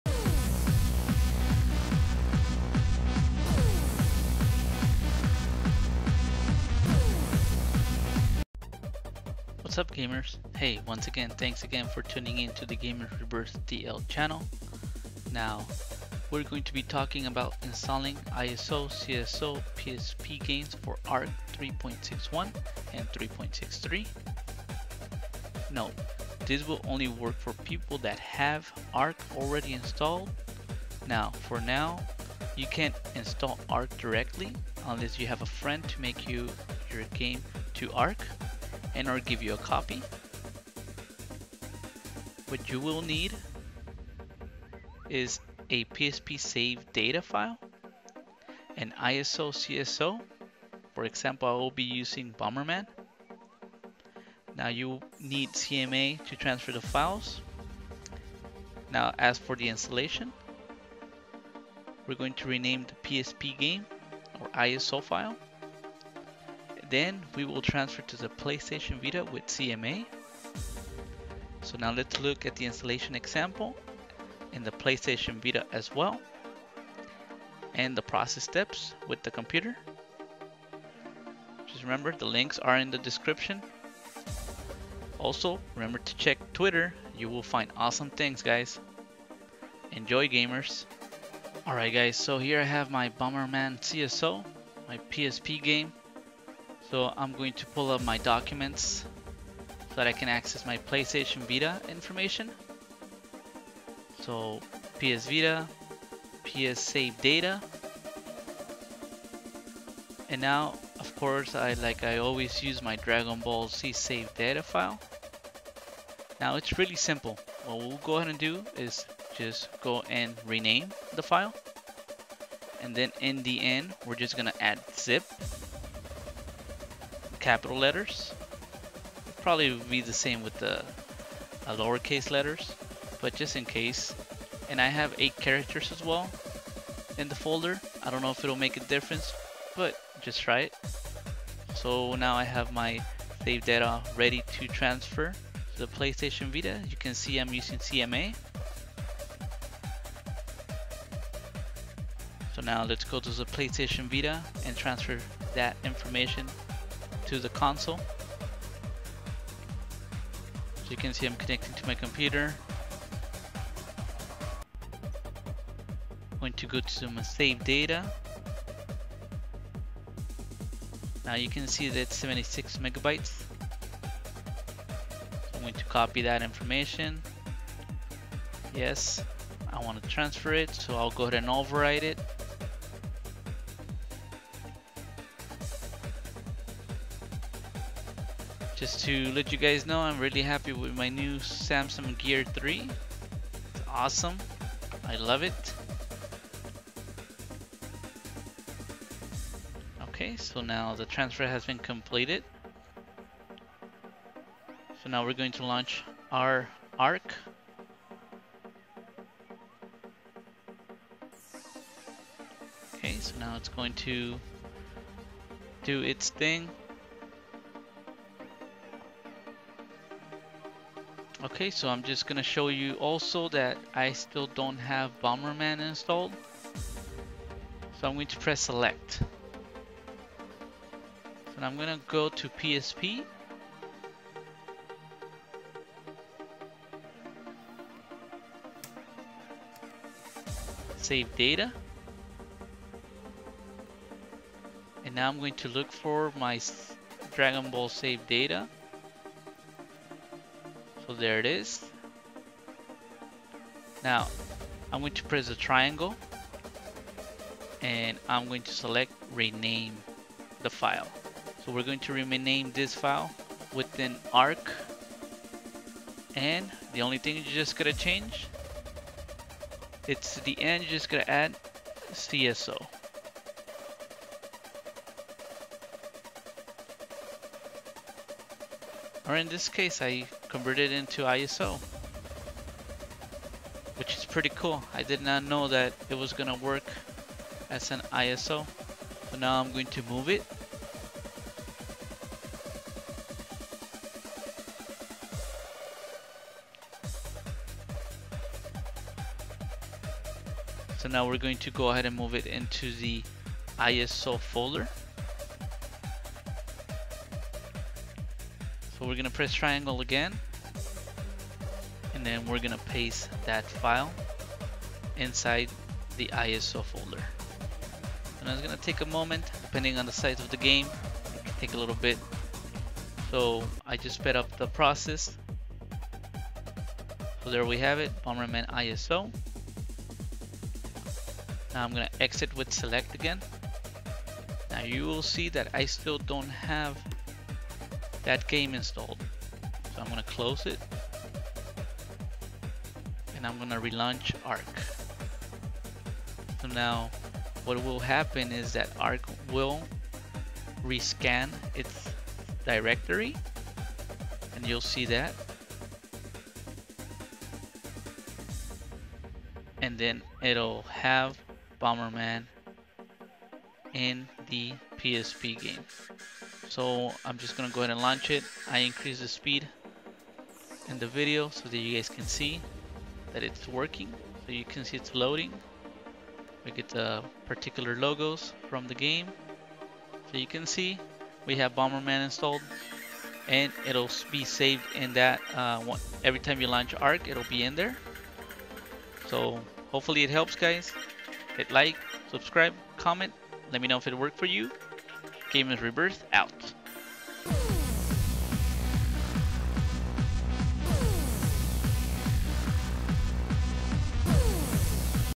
Installing ISO/CSO PSP Games for ARK 3.61 & 3.63! EASY Installation Guide!
Installing ISO/CSO PSP Games for ARK 3.61 & 3.63!!!*
Note: This will work for people that have ARK already Installed. Now for now you can’t Install ARK directly unless you have a friend to make your game to ARK and/or give you a copy!
You will need:
A PSP SaveData File
An ISO/CSO (I will be using BomberMan)
CMA to transfer the files
Installation:
Rename your PSP Game
Then we will transfer to the Vita with CMA
lets look at the Installation example in the PS Vita!
P.S. Check Twitter you will find awesome things
Enjoy Gamers!!!